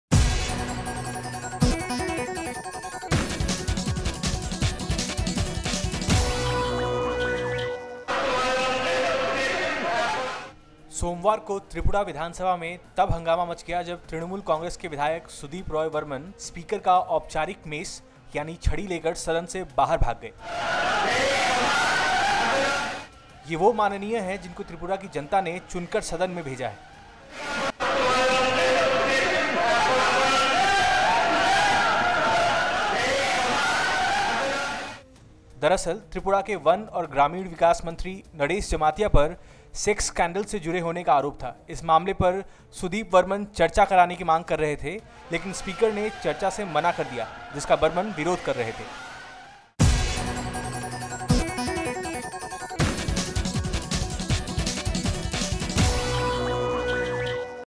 त्रिपुरा विधानसभा में विधायक ने लगाई दौड़ I Watch now Crime world Show
सोमवार को त्रिपुरा विधानसभा में तब हंगामा मच गया जब तृणमूल कांग्रेस के विधायक सुदीप रॉय बर्मन स्पीकर का औपचारिक मेस यानी छड़ी लेकर सदन से बाहर भाग गए। surag || Doordarshan || Number one || Detective || Suspense || Thrillers ||
Sudesh beri || || actor || murder mystery || # best serial